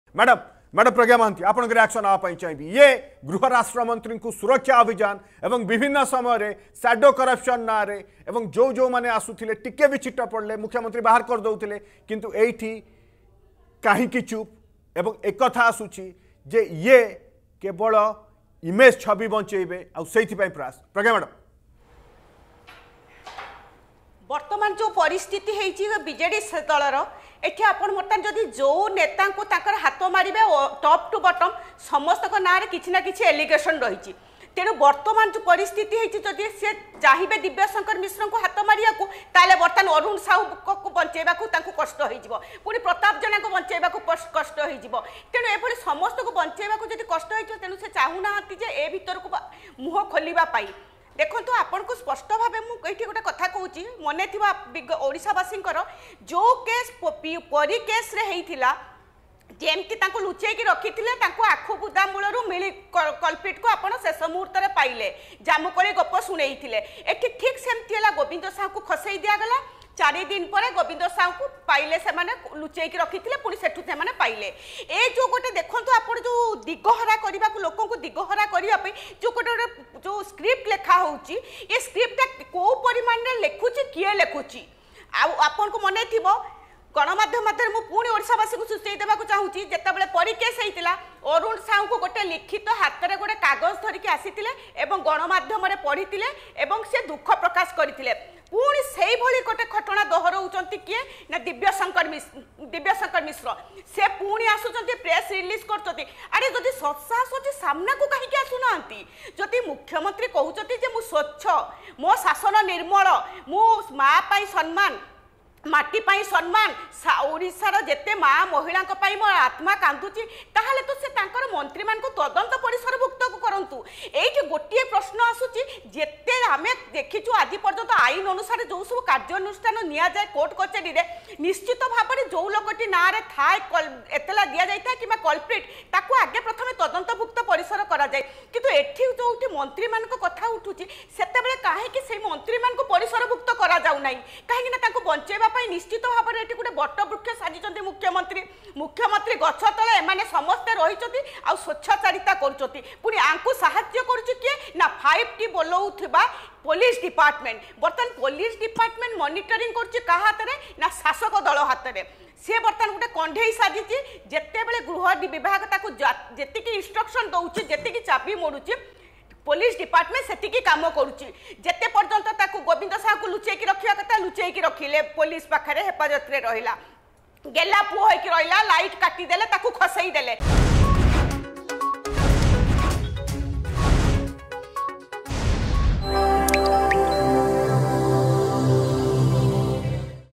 ମୁଖ୍ୟମନ୍ତ୍ରୀଙ୍କ ଛତ୍ରଛାୟାରେ ବିଜେଡିର ଦାଗୀ ନେତା
ମନ୍ତ୍ରୀଙ୍କୁ ତଦନ୍ତ ପରିସରଭୁକ୍ତ କରାଯାଉନି,ମନ୍ତ୍ରୀଙ୍କୁ ବଞ୍ଚେଇବା ପାଇଁ ବଟବୃକ୍ଷ ସାଜିଛନ୍ତି ମୁଖ୍ୟମନ୍ତ୍ରୀ : ପ୍ରଜ୍ଞା ମହାନ୍ତି,କଂଗ୍ରେସ ନେତ୍ରୀ